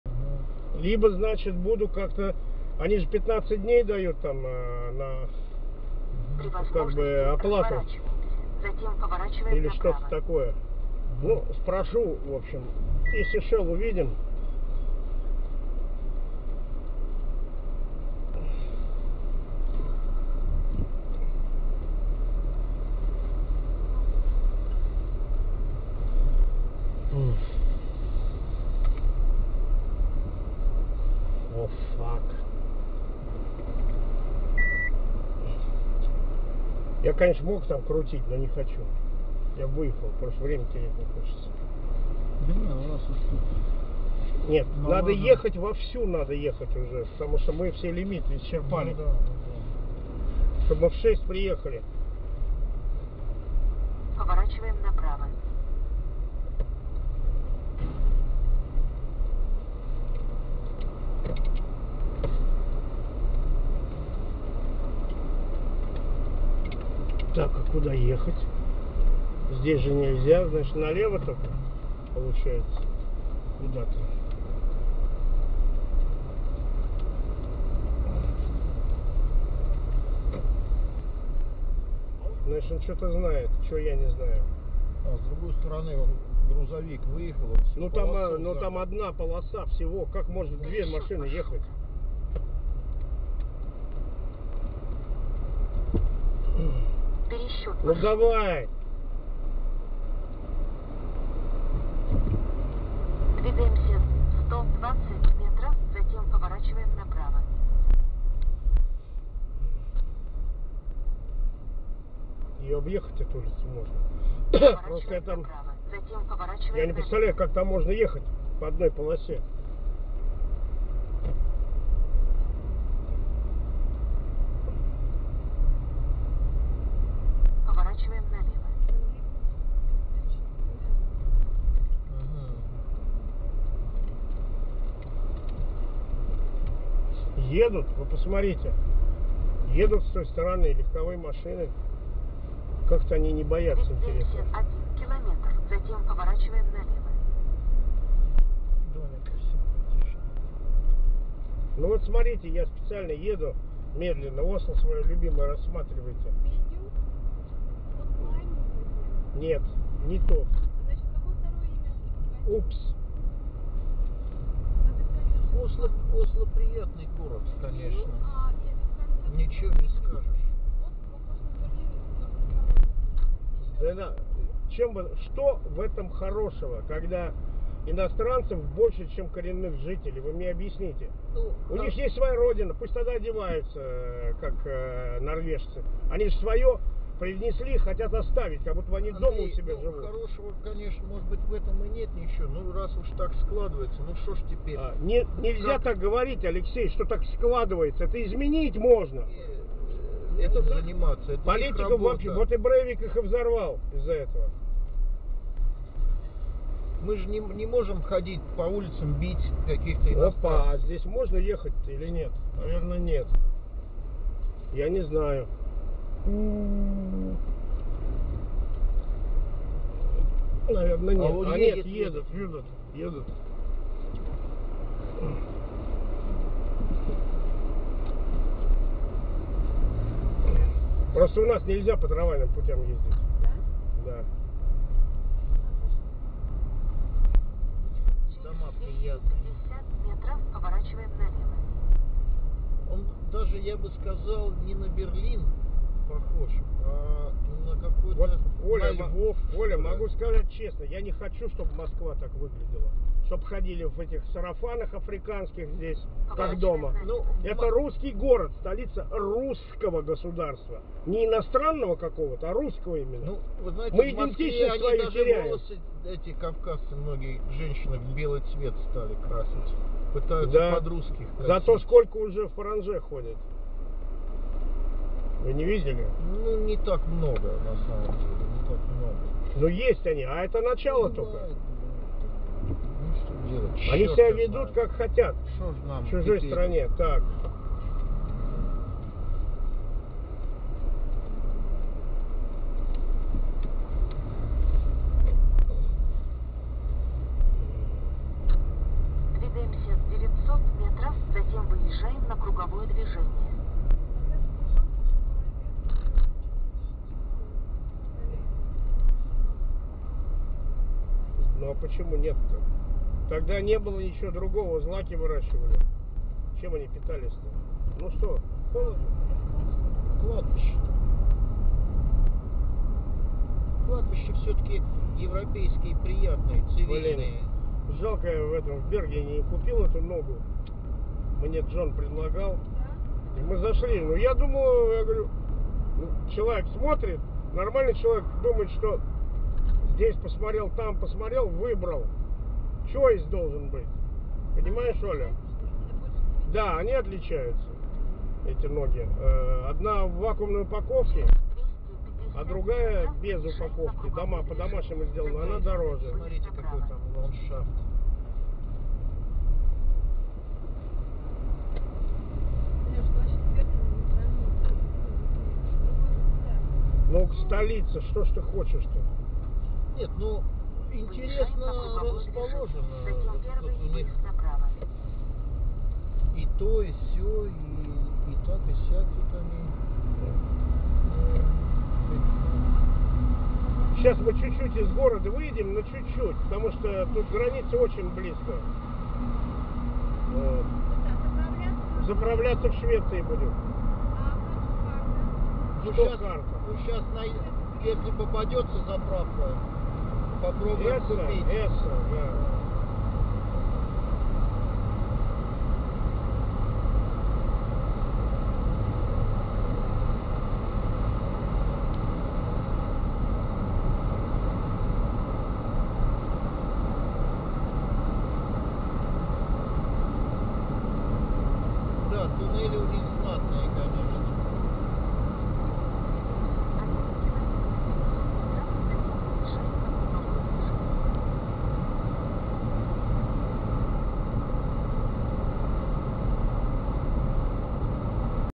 ДОРОГА ОСЛО - СТОКГОЛЬМ / OSLO - STOCKHOLM ROAD (1)
Запись с видеорегистратора.
Начало путешествия в Стокгольм.
10 августа 2012,